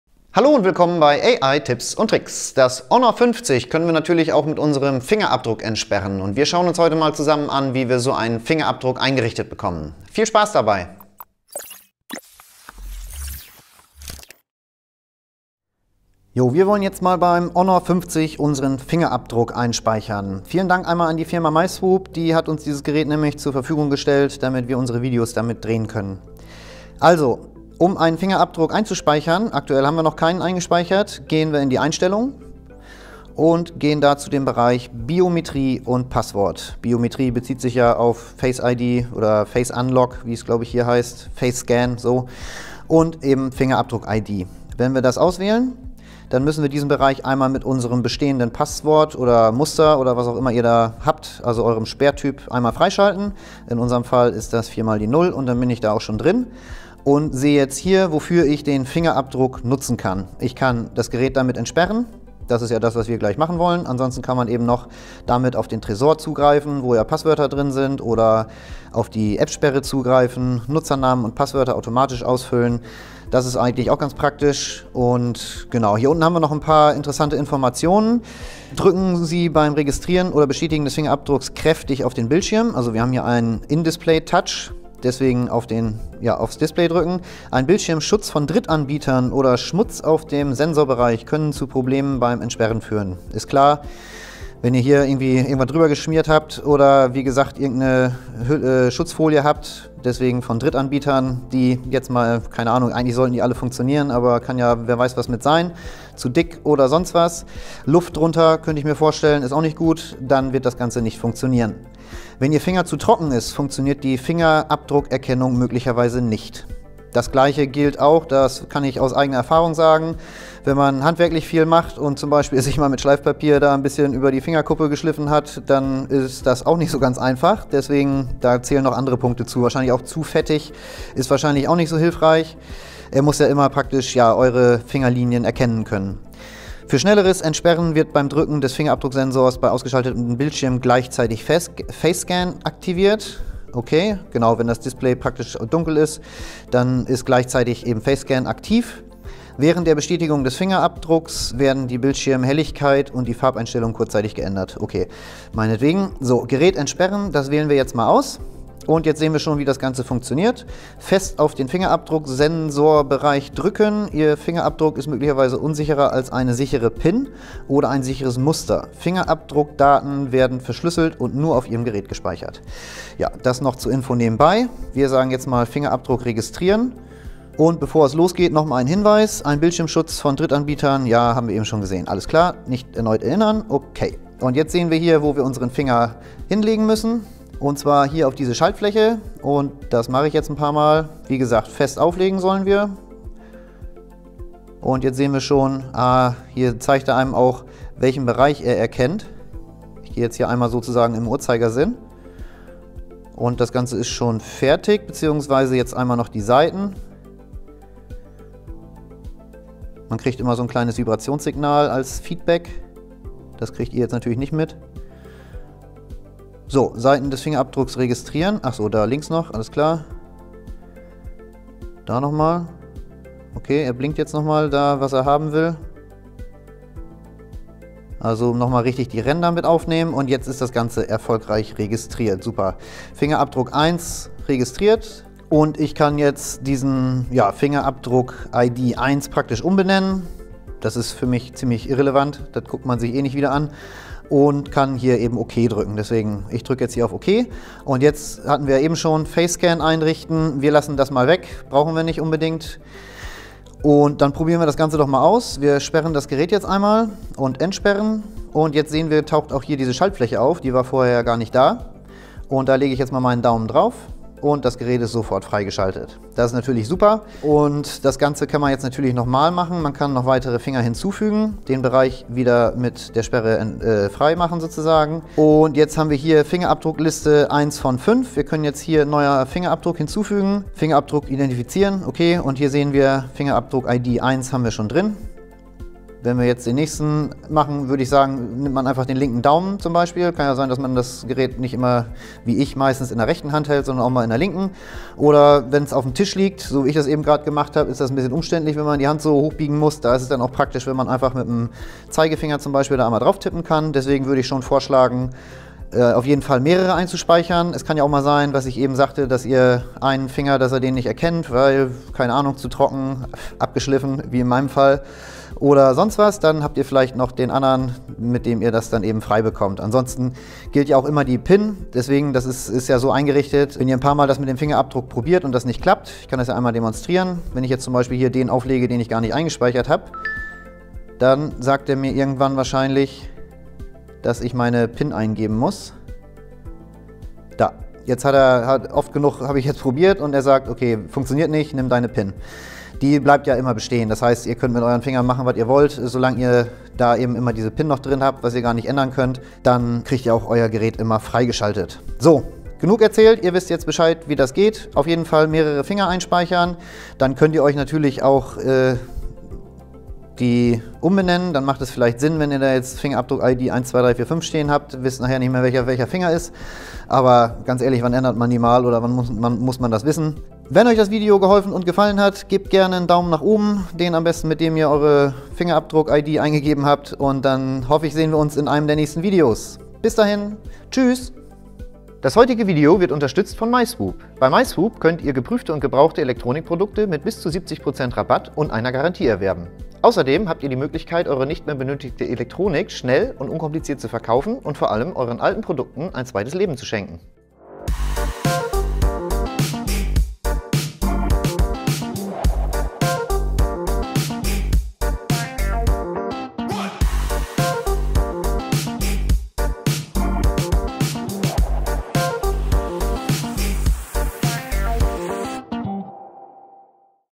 Honor 50 - Fingerabdruck einrichten • 📱 • 👆🏼 • 🔐 • Anleitung | Tutorial
Auch bei Eurem Honor 50 könnt Ihr natürlich einen Fingerabdruck einrichten, um Euer Gerät schnell und einfach zu entsperren. Das Honor 50 erkennt Euer Daktylogramm und schaltet sich für die Nutzung von Euch frei. So geht Ihr sicher, dass niemand sonst Euer Handy entsperren kann. Wir zeigen Euch hier, wie das mit dem Einrichten Eures Fingerabdrucks am Honor 50 funktioniert. Für Eure Sicherheit. Noch ein Tipp: unbedingt mehrere Finger "einspeichern", damit Ihr immer eine Alternative beim Entsperren habt, falls Ihr z.B. mal einen Finger verloren oder zu Hause vergessen habt... Viel Spaß beim Video und beim Einrichten Eures Fingerabdrucks.